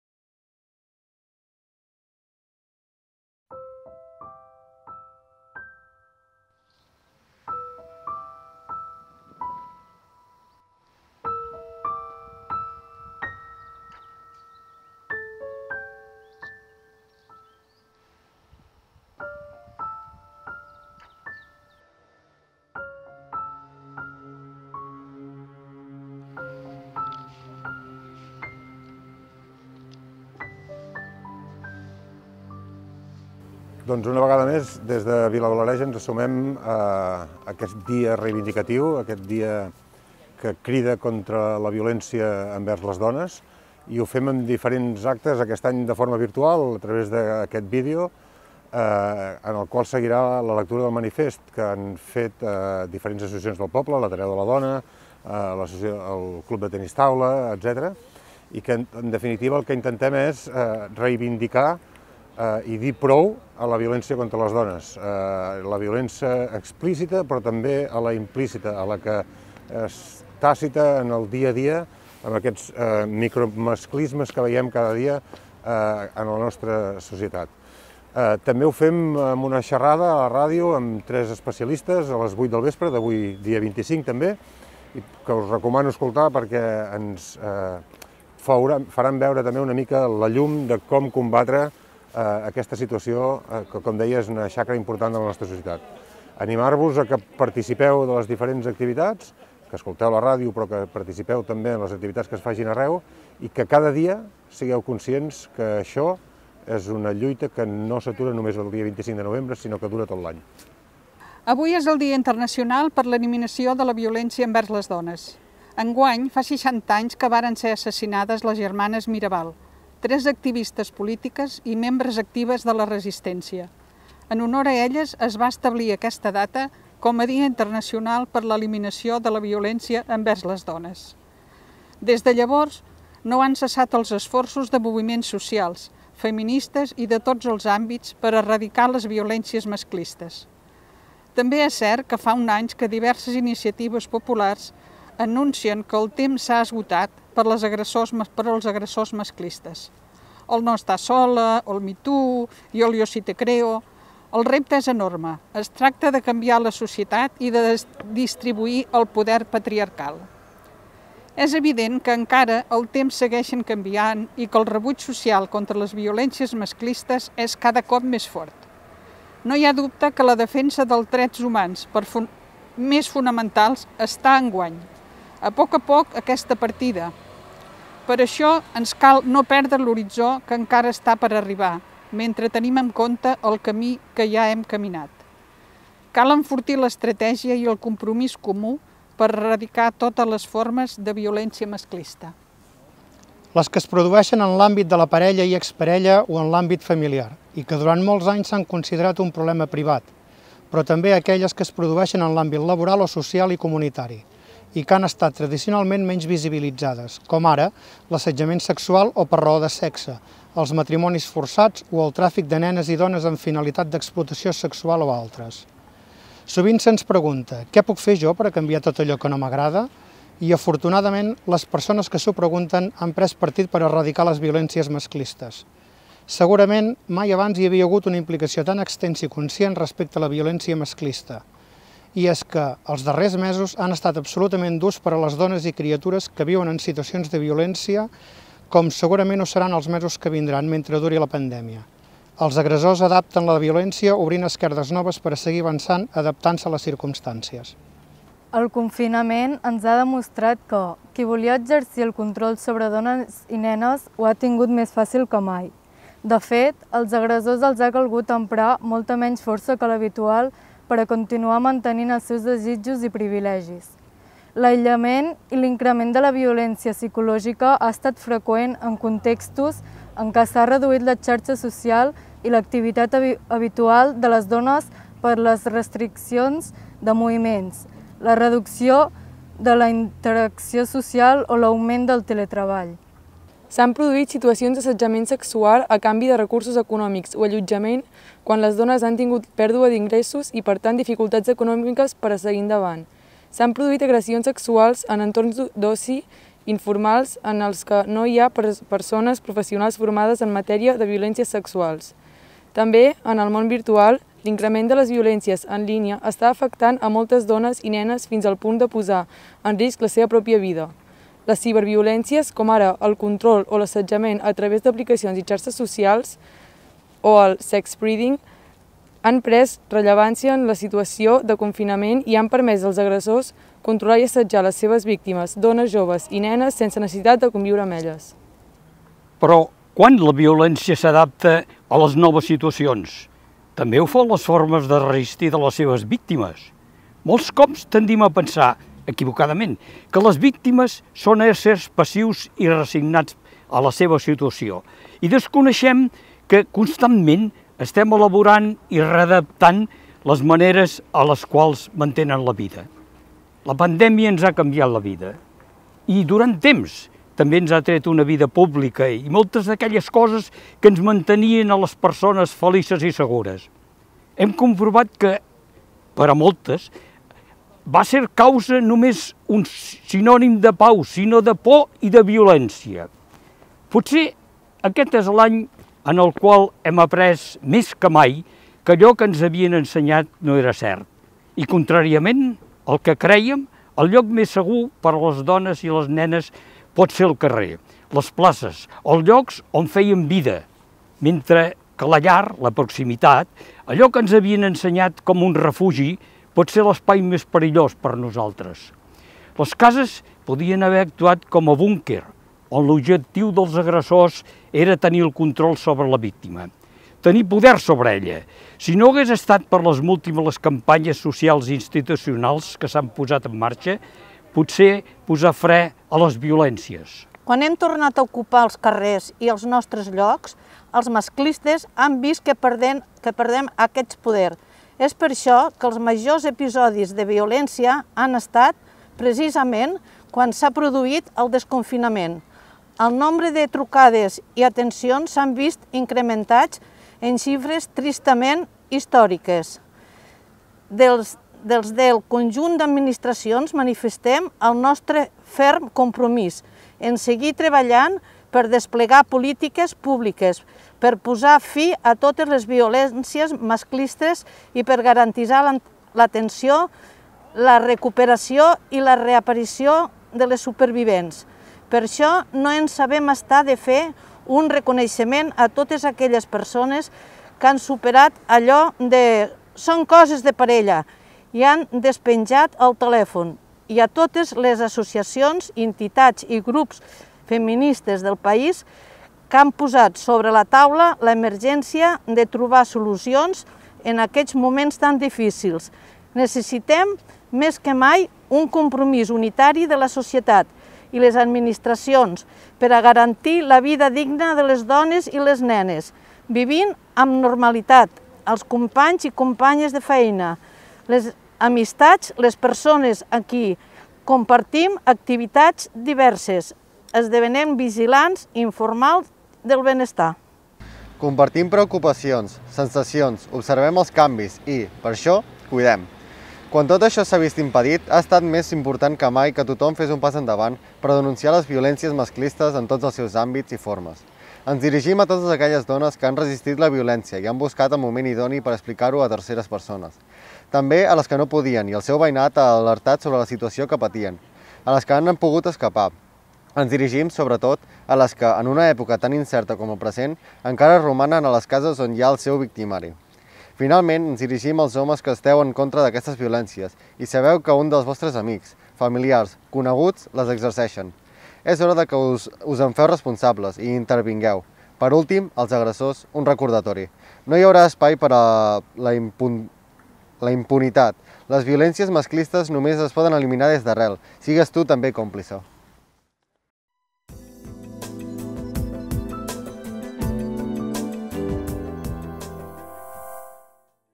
Acte del 25N a Vilablareix
A Vilablareix commemorem el Dia Internacional per a l'Eliminació de la Violència envers les dones amb la lectura del manifest, a càrrec de veïns i entitats del poble, una exposició a plaça Catalunya, i un programa especial que s'emet per Ràdio Vilablareix 107.5FM